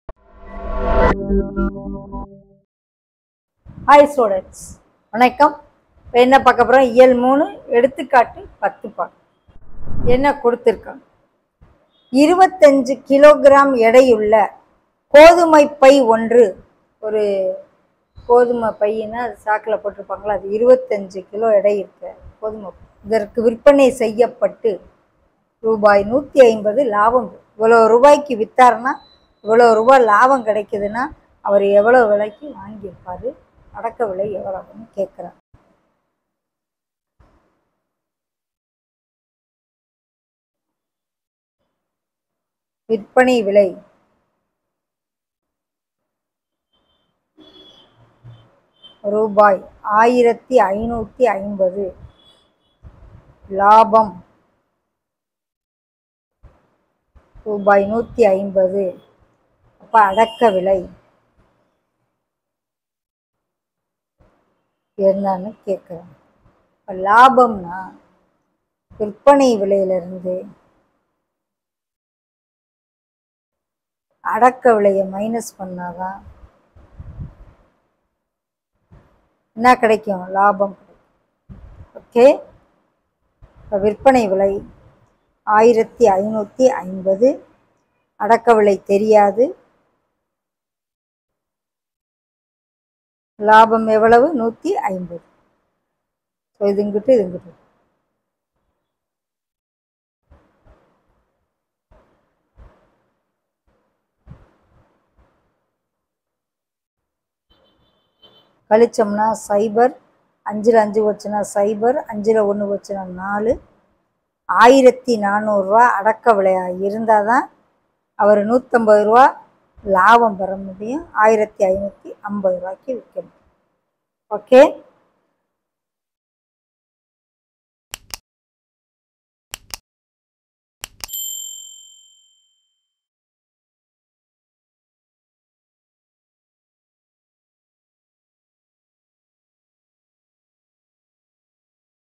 6th Maths | Term2 | Chapter 3 | Example 10 | Tamil Medium | TNPSC | All Solved | Ranjitham Maths
In this Video Maths Teacher, Ms. Ranjitham Solves all Questions and Answers in 6th Maths Chapter 3.